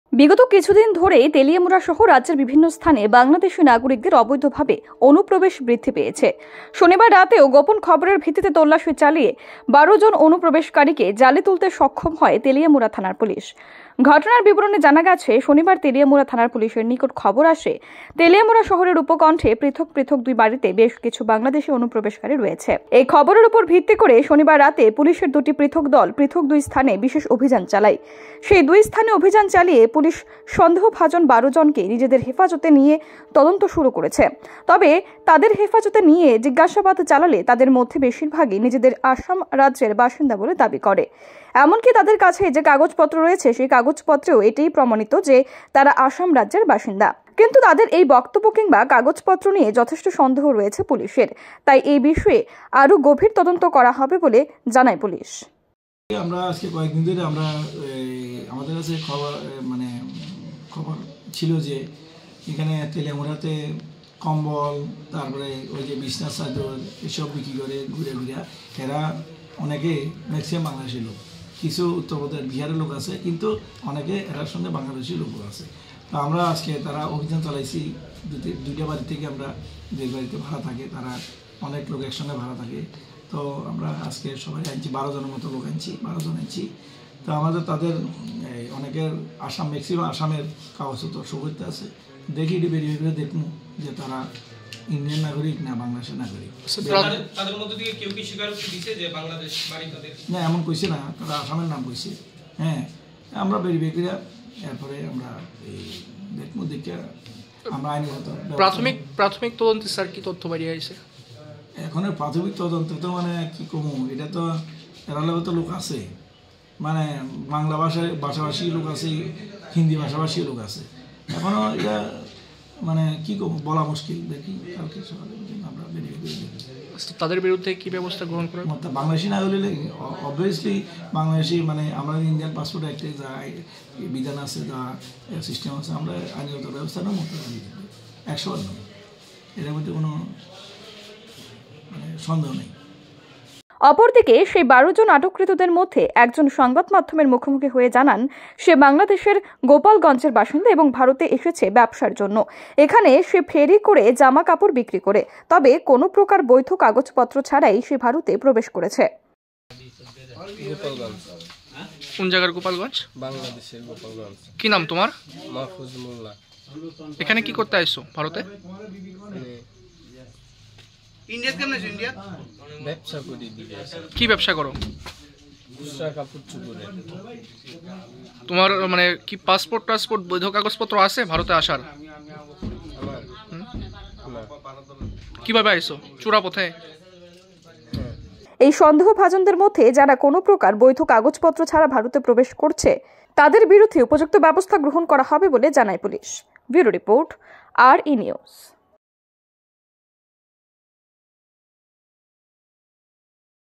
১২ জন অবৈধ অনুপ্রবেশকারীকে জালে তুলতে সক্ষম হয় পুলিশ
১২ জন অবৈধ অনুপ্রবেশকারীকে জালে তুলতে সক্ষম হয় পুলিশ